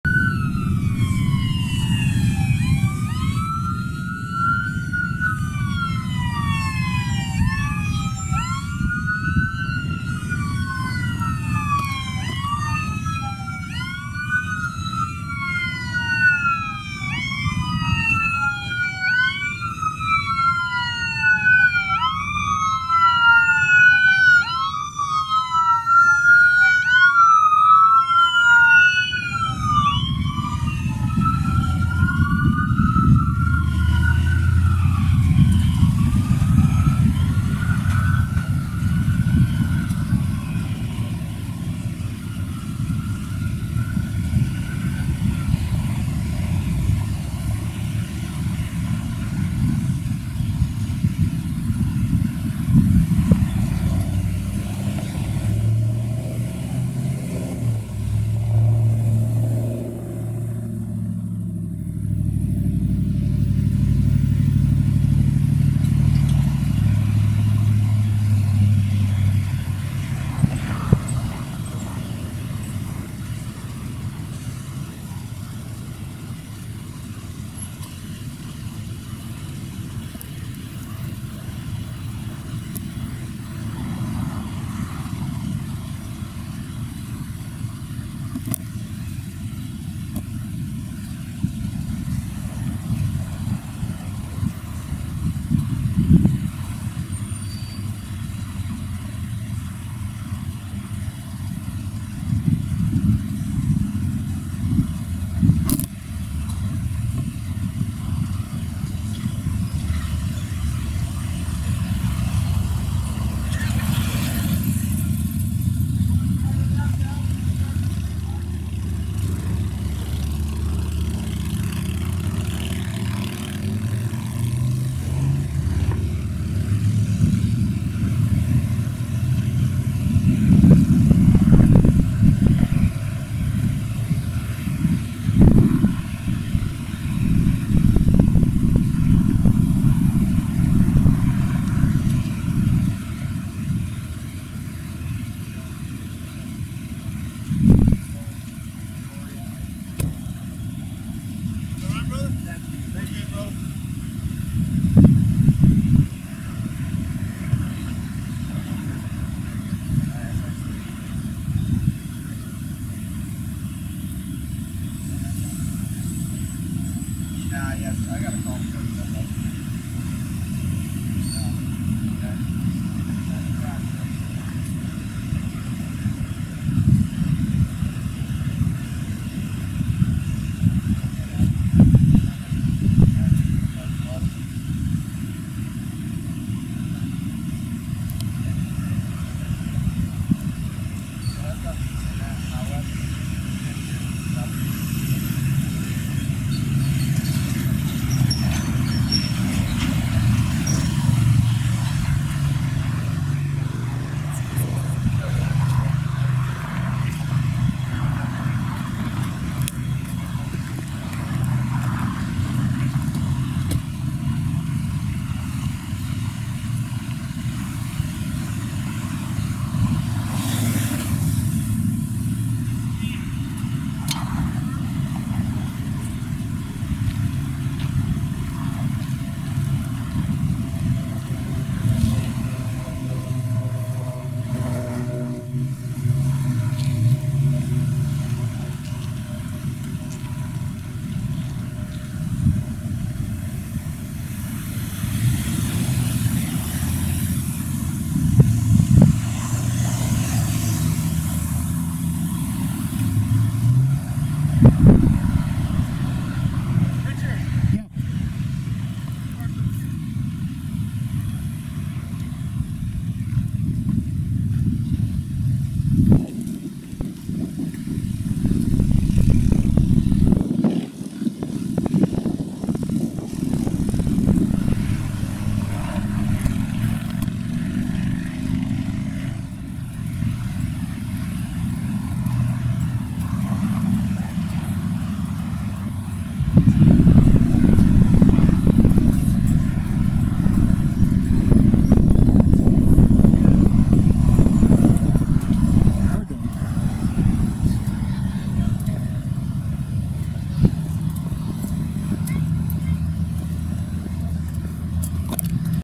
DPS Traffic stop, 7th and Chicon, Austin TX 3/10/18
Fairly routine traffic stop.  DPS seems to be over-patrolling East Austin during SXSW.